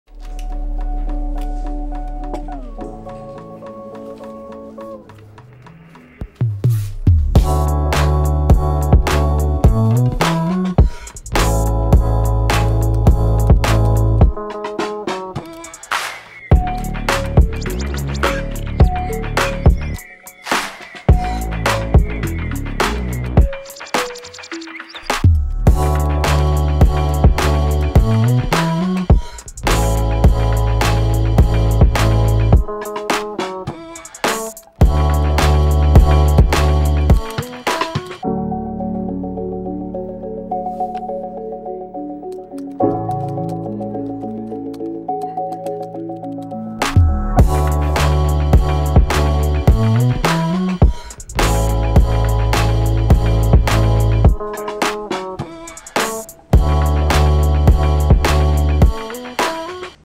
ERGO K860 Ergonomic Split Keyboard
Type more naturally with ERGO K860—an Ergonomic Split Keyboard that promotes a more relaxed typing posture, reduces muscle strain, and provides more wrist support.